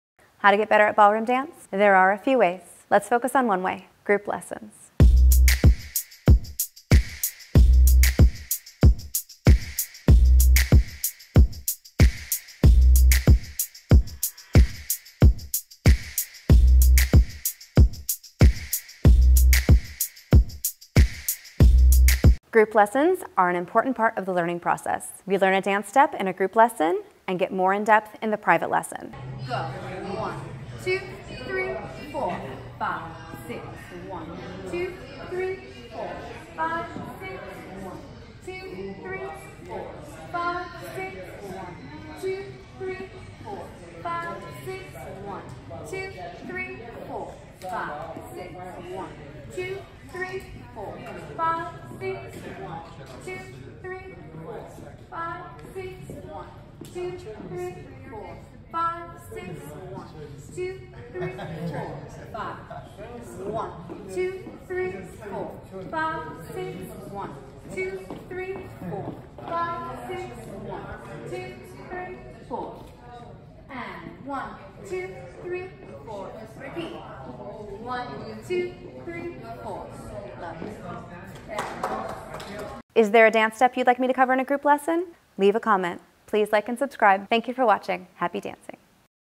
WALTZ|how to|BALLROOM GROUP LESSON
Hollywood Academy of Dance
Van Nuys, CA

American Waltz;
traveling step
fifth position
traveling
open break and underarm turn
traveling steps
promenade twinkle to running underarm turn